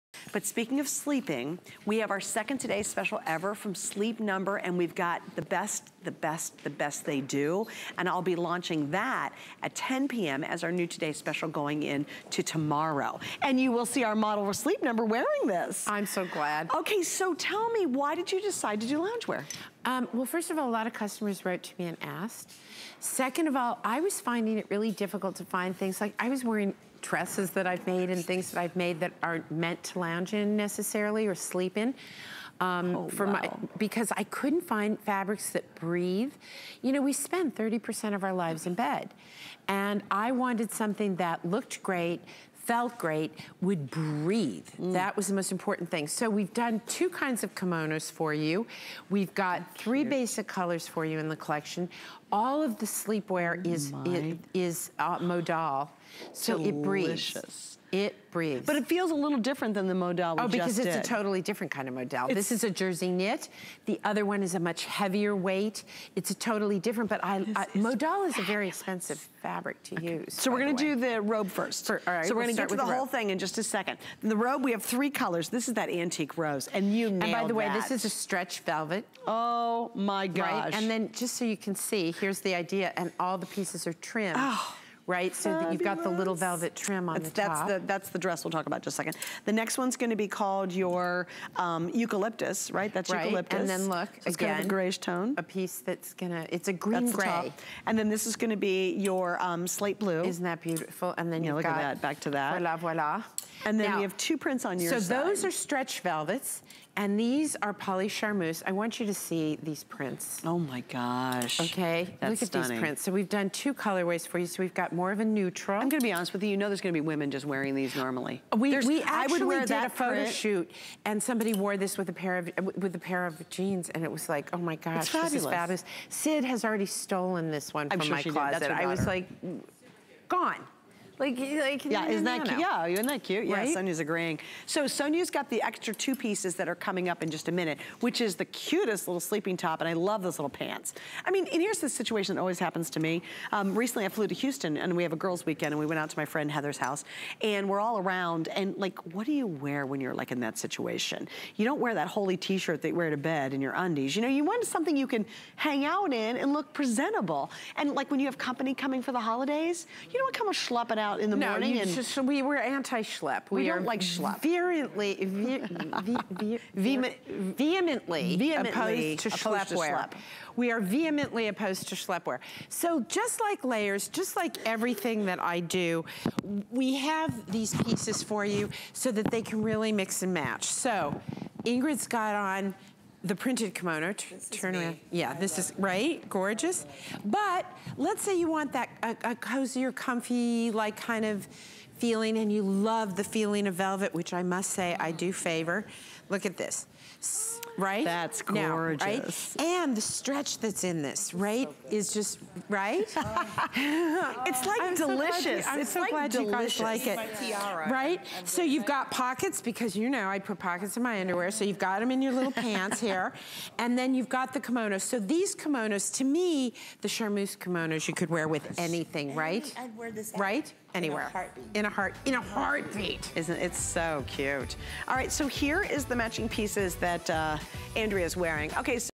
WynneLounge Knit Velvet Kimono Robe
Extend the life of your fall wardrobe. This robe is an easy topper for those transitional seasons, so top it over tanks in late summer and early fall and over...